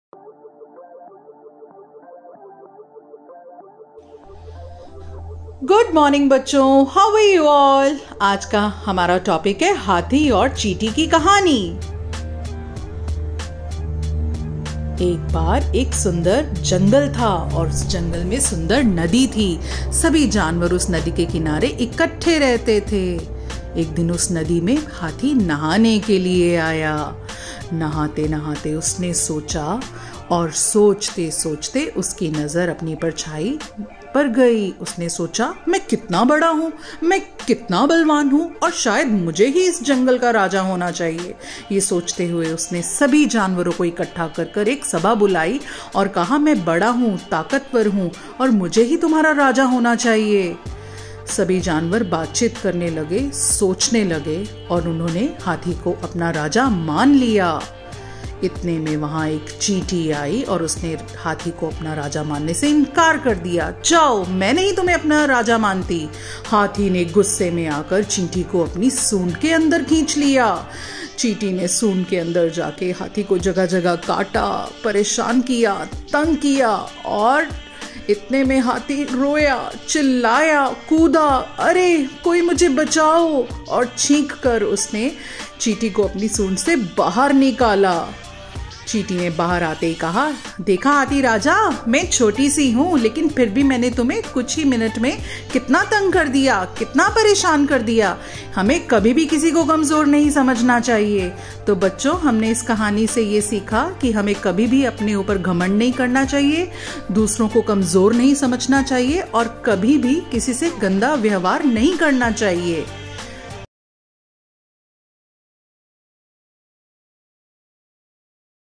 Story Telling | Hatthi aur Chinti | Pre Primary | Kids
After this video you will able to learn
1. not be over confident
2. we should be humble
3. Development of Listening Skills

Made by: Manika Sharma
School: Government Model Senior Secondary School Sector 10, Chandigarh